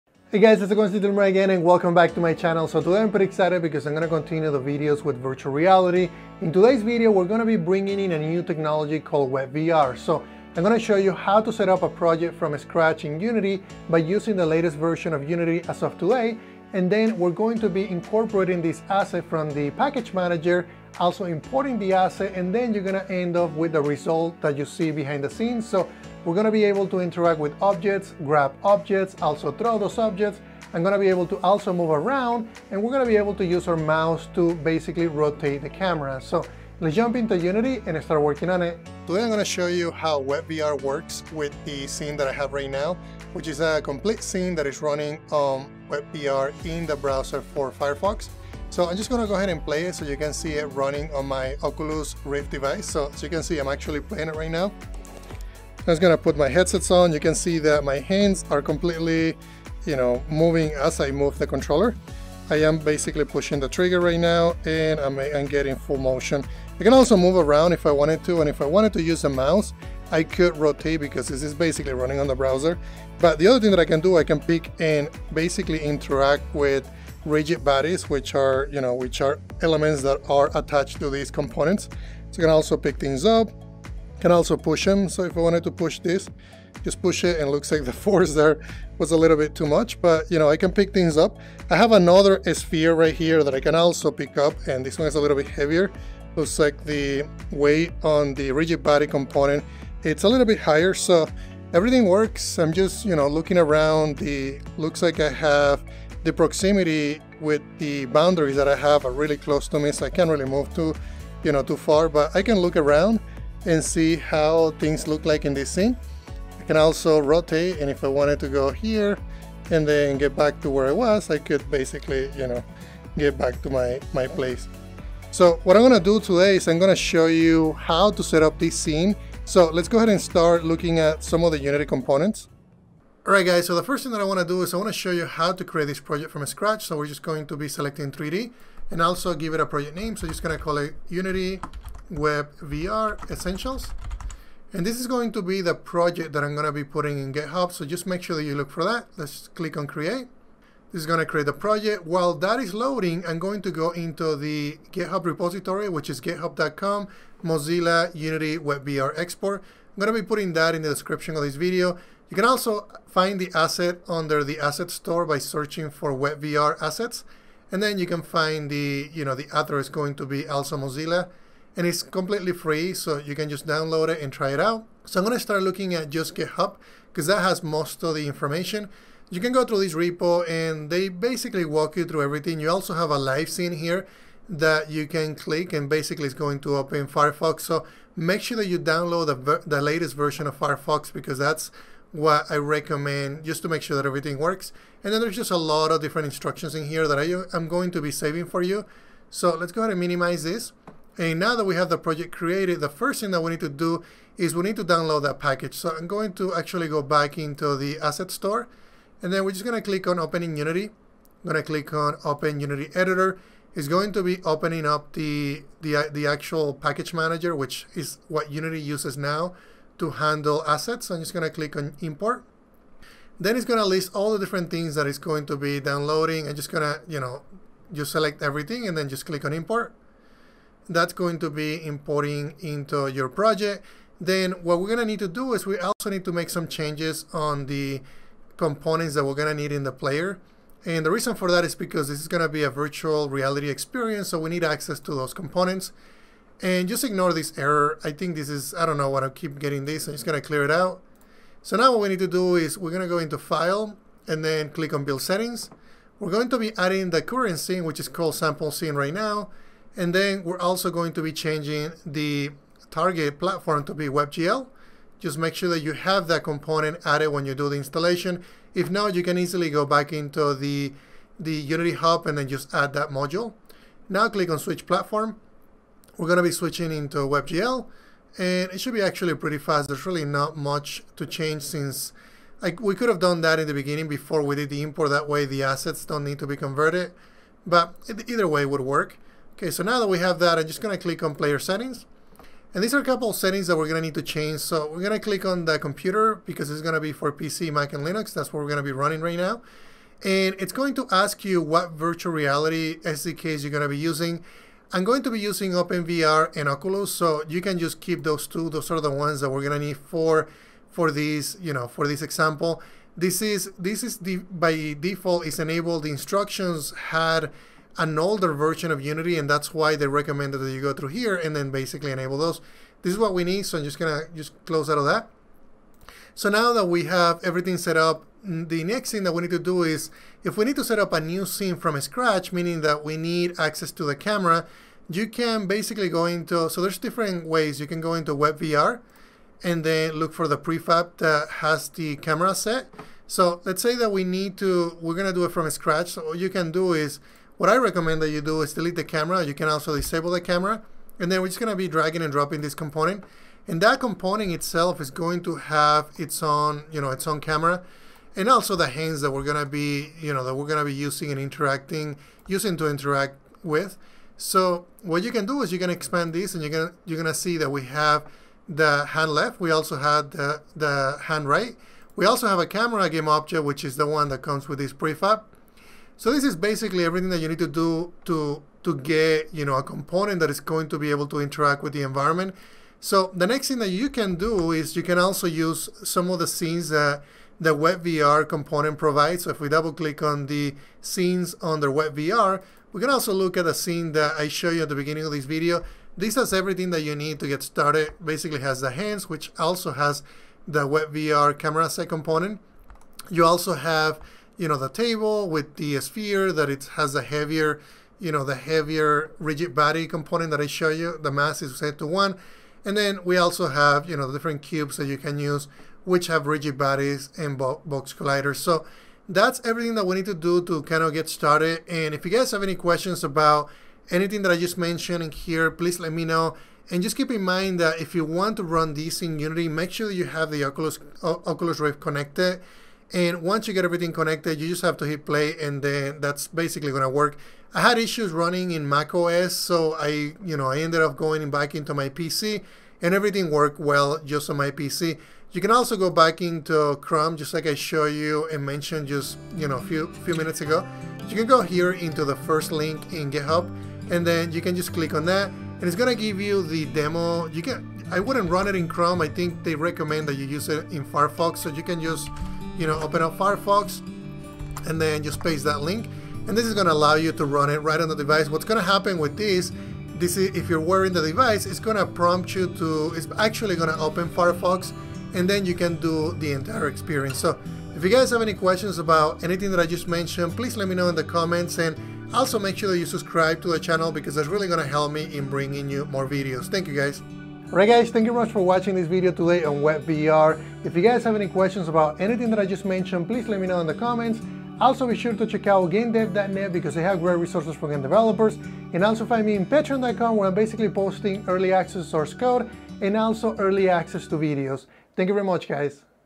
Unity WebVR - How To Setup WebVR In Unity3d With WebGL?
Today I like to show you how to use WebVR in Unity by experimenting with an asset developed by the Mozilla team for Unity3d. The asset used in this video is integrated into a WebGL project and I walk you through how import the asset as well as how to configure the project to run in a virtual reality device (Oculus Rift).